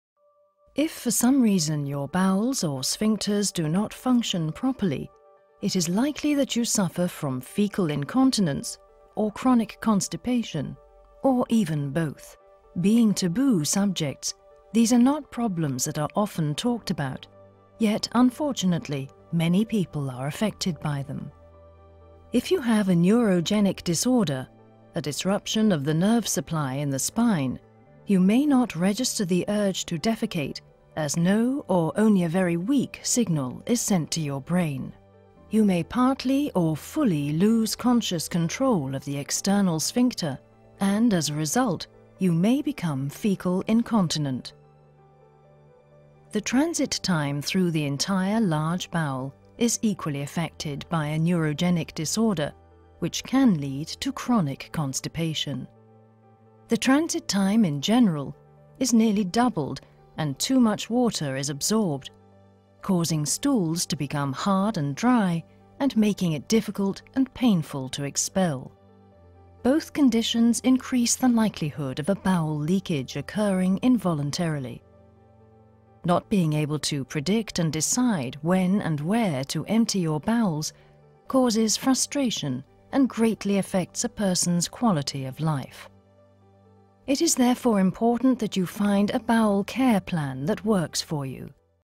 Bowel incontinence and chronic constipation
A video animation introducing the conditions of bowel incontinence and chronic constipation.